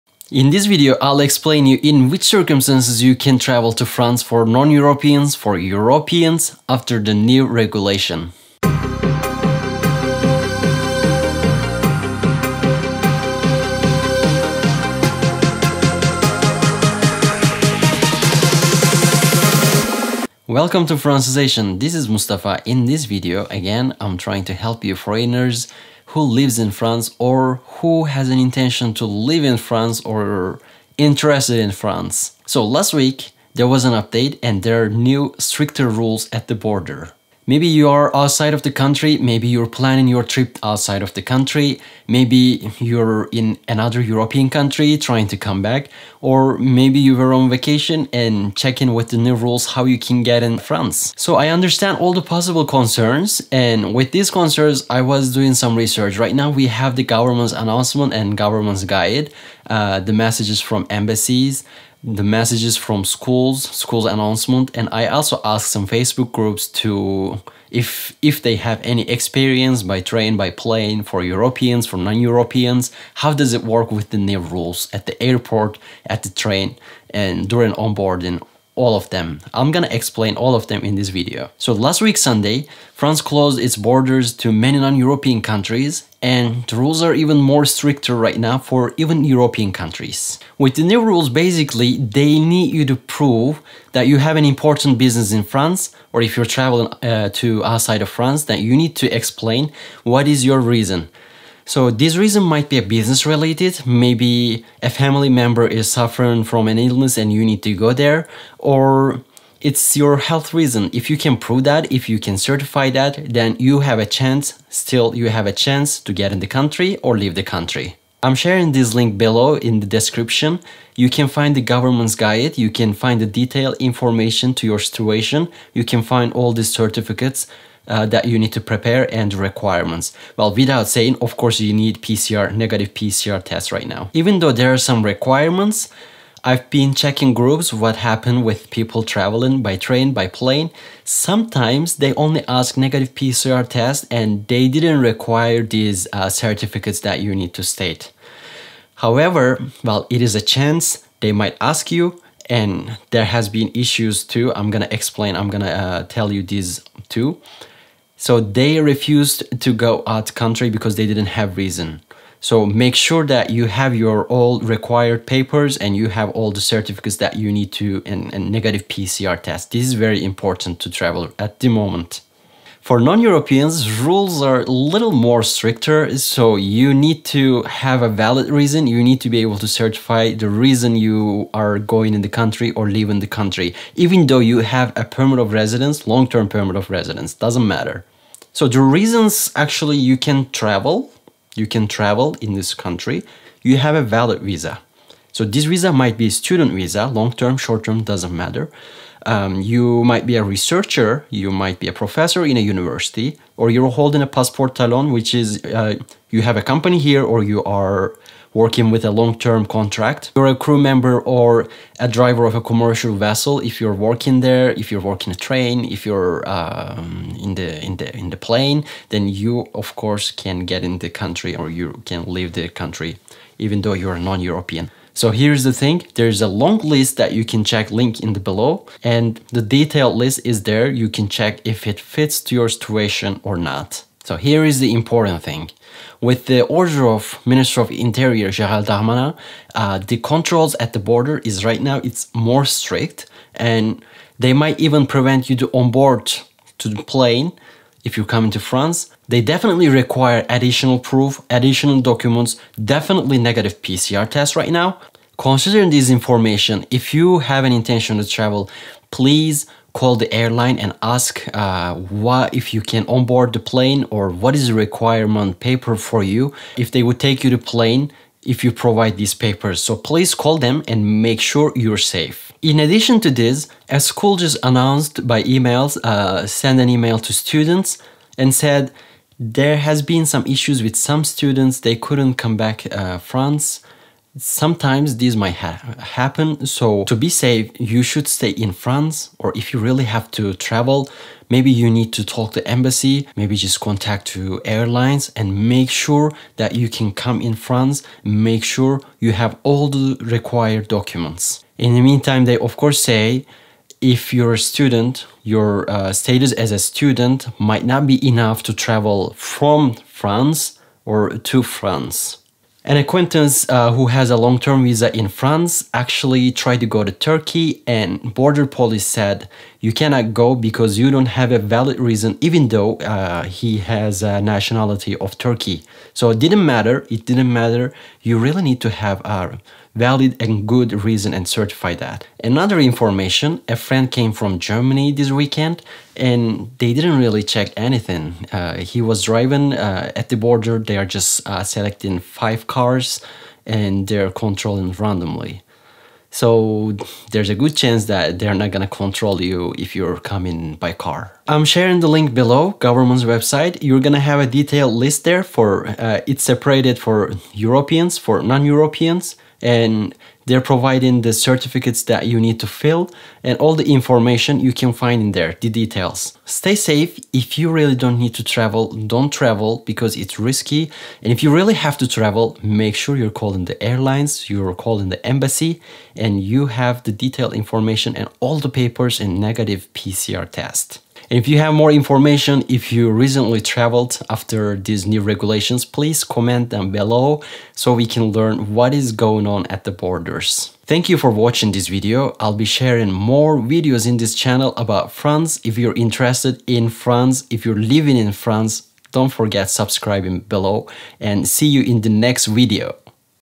The new COVID19 rules at the border of France
As of the beginning of February, France closed its borders to almost all travel from outside the EU, but also tightened the rules on travel from inside Europe.

I explain the rules and share some experience from travellers in this video.

Comment if any of what I tell happened to you or share your experience with us!